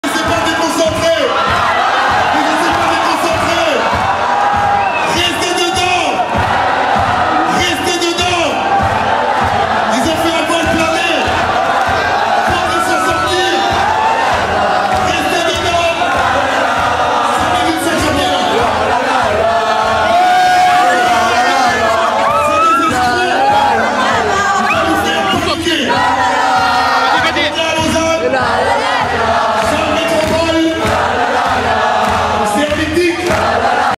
YOUSSOUPHA CONCERT DE LA MÉTROPOLE - S-PI ET SAM'S SAUTENT DANS LA FOULE EN DELIRE
Spi et sams sautent dans le public de la MÉTROPOLE !